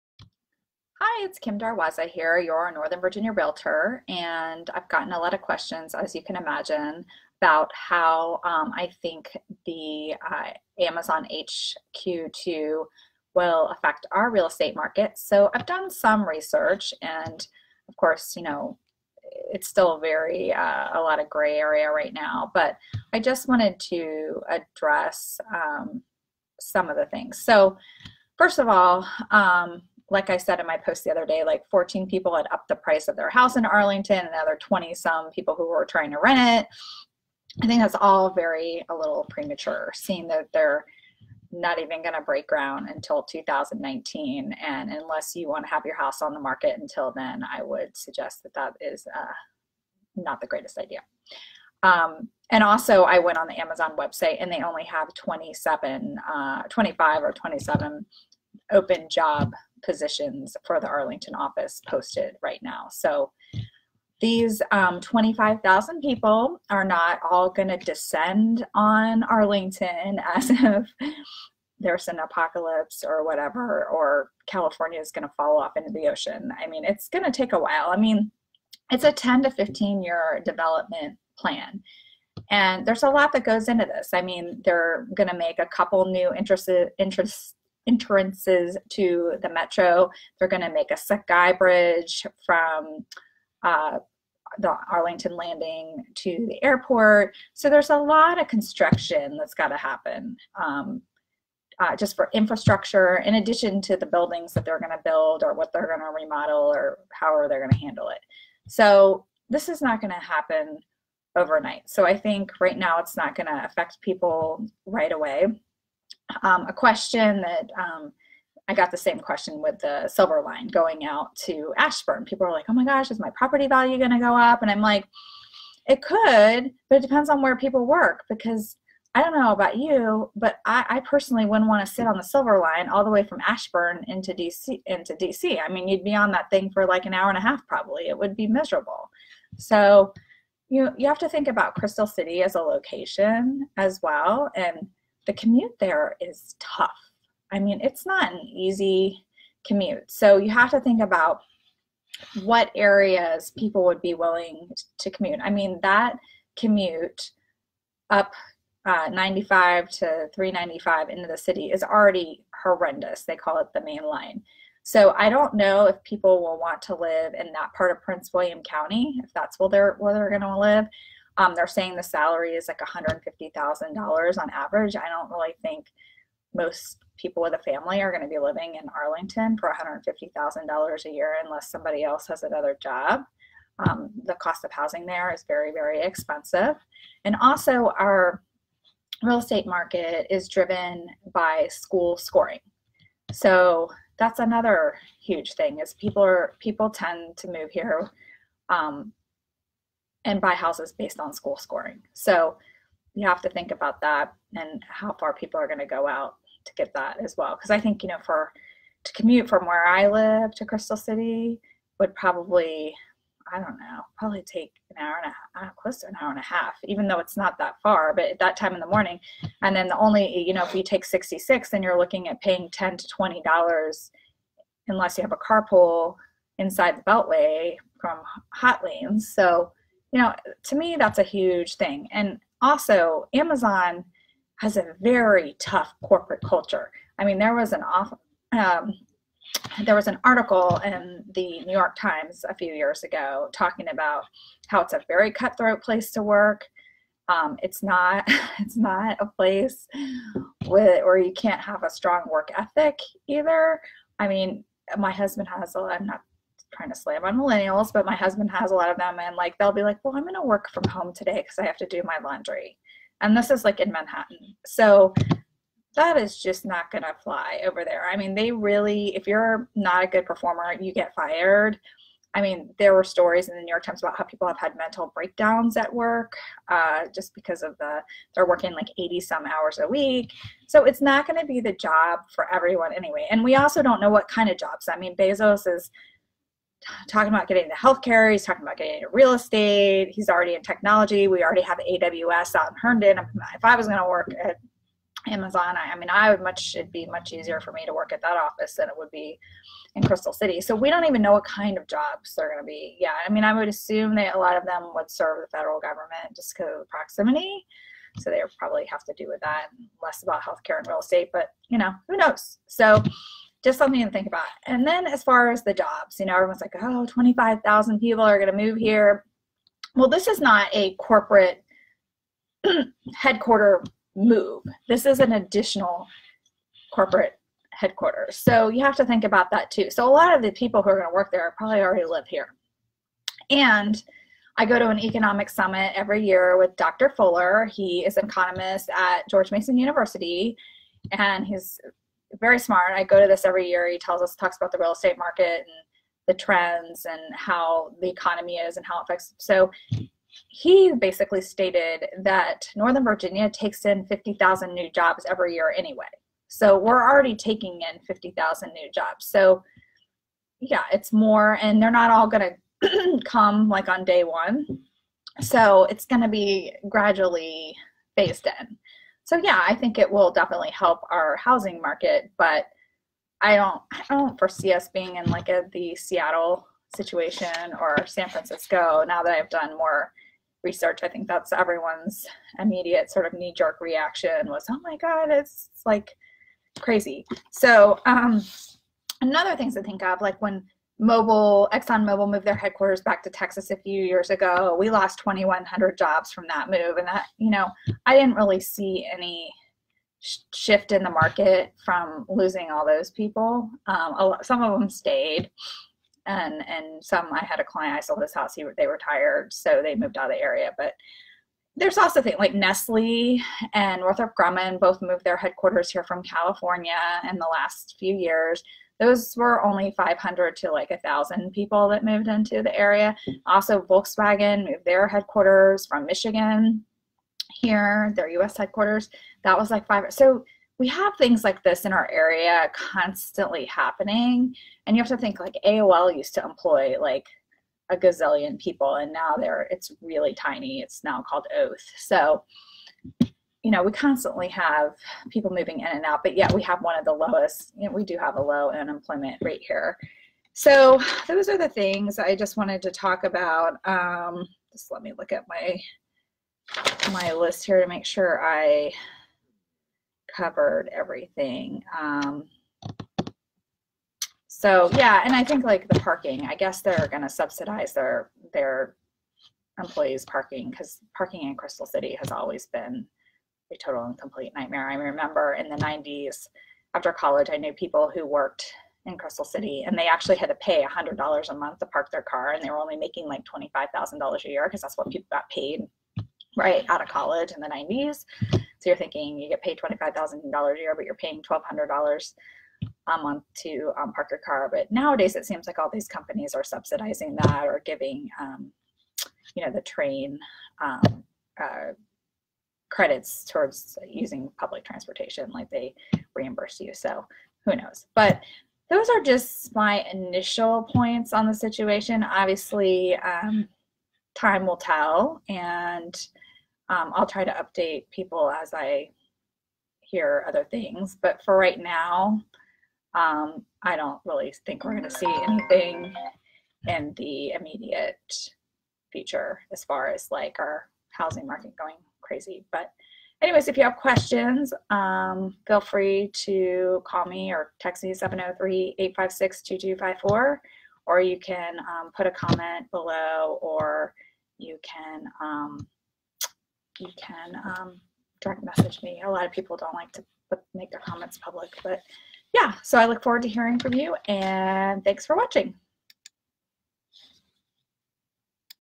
Amazon HQ2 and Real Estate
How I think the Northern Virginia Real Estate Market will be affected by Amazon HQ2 moving into Arlington.